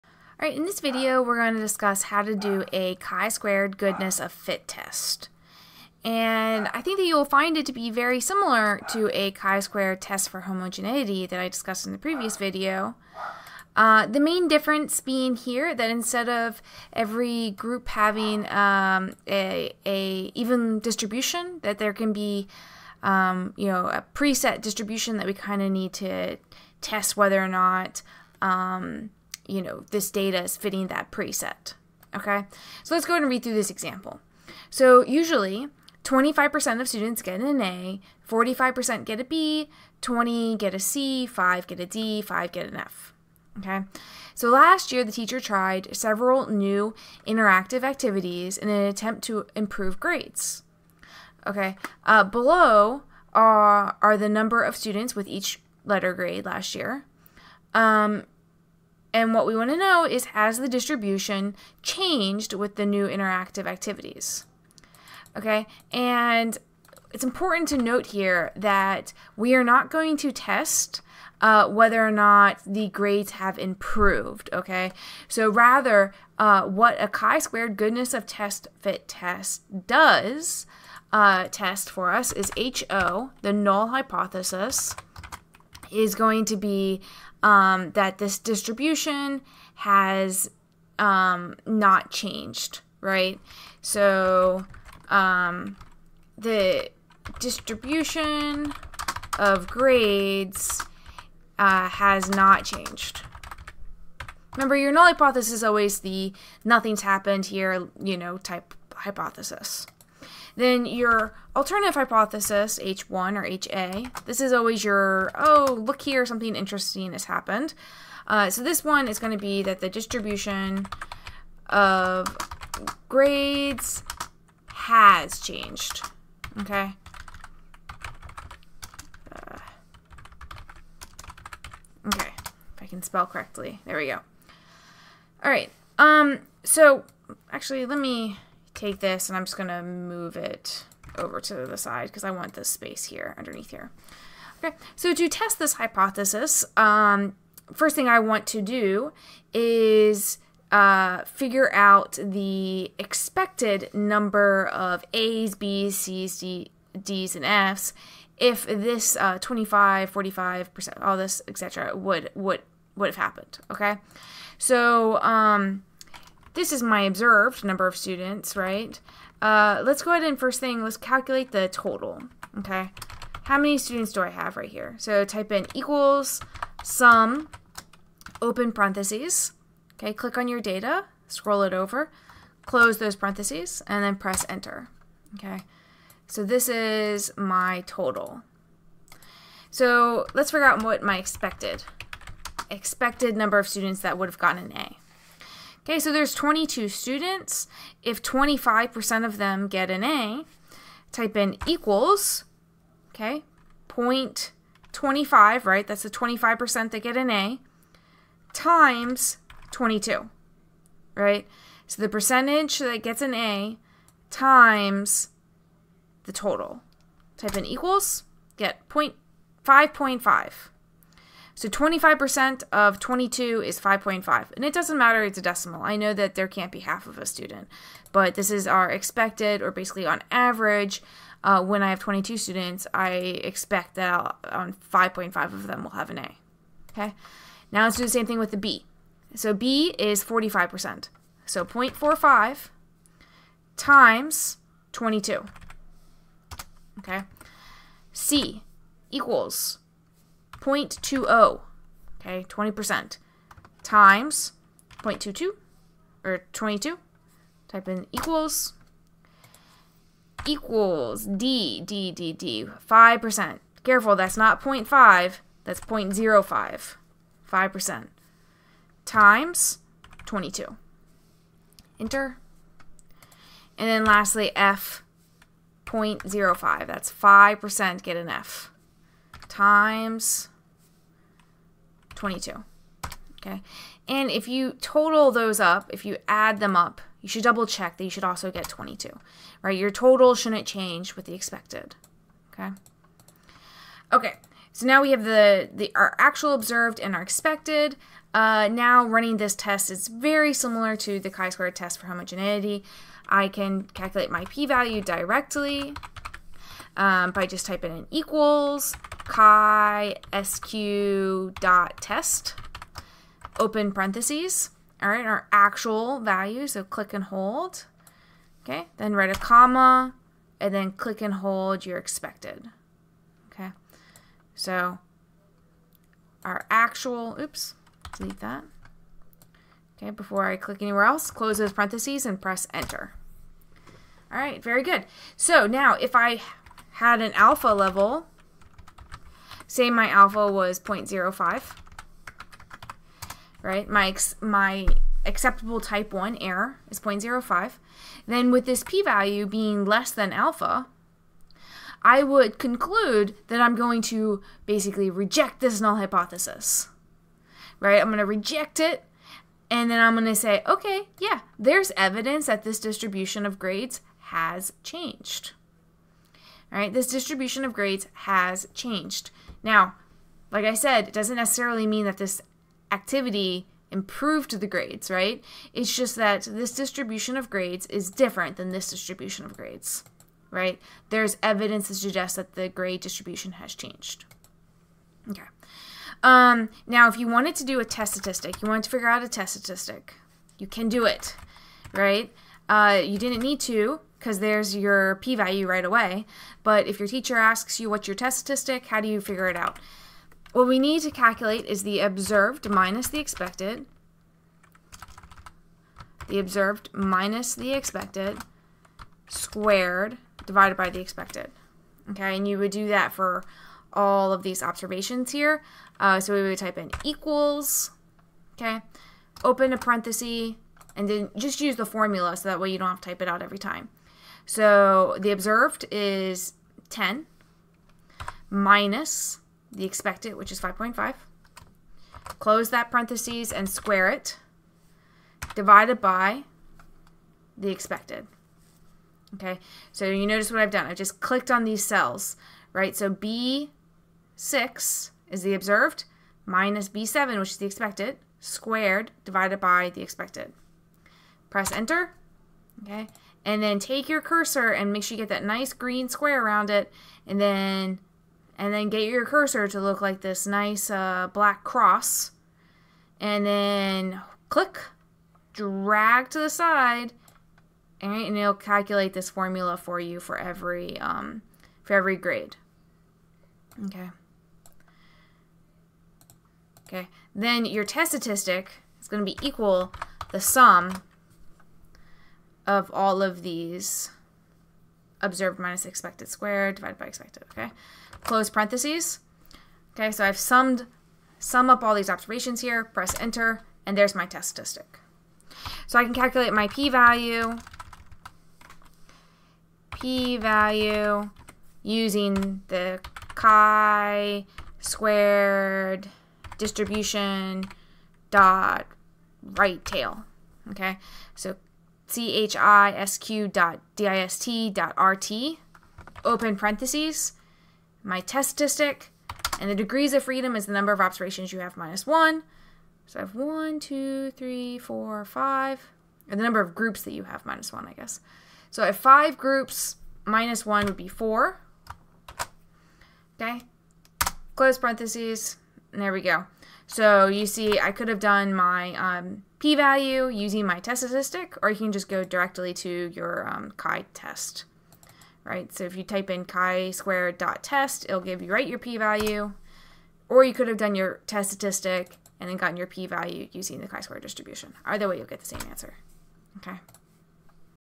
Chi Squared Goodness of Fit Test In Excel
Learn how to use =CHISQ.TEST and =CHISQ.DIST.RT